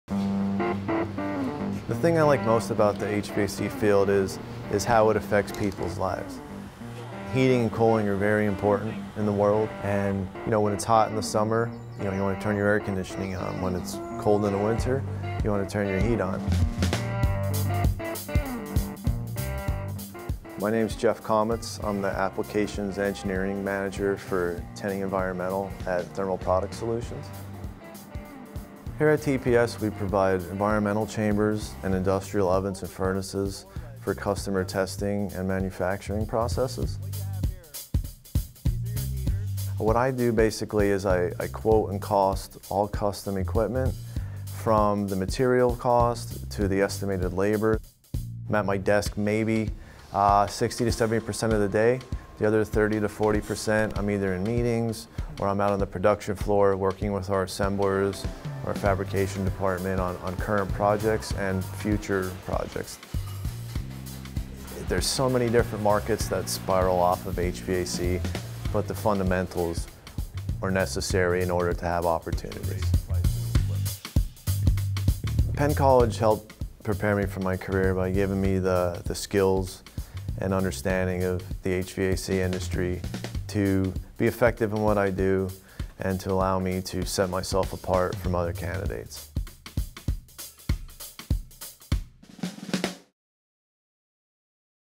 HVAC with Jeff Comitz
Meet Jeff Comitz, Applications Engineering Manager at Thermal Product Solutions (TPS). After earning his bachelor’s degree in Heating, Ventilation & Air Conditioning Design Technology at Penn College, Jeff accepted an offer at TPS where he partners with global organizations to develop industrial ovens and furnaces for testing and manufacturing processes. Take a look inside Jeff’s career. See how he applies his Penn College education every day. And imagine yourself pursuing a future made by hand.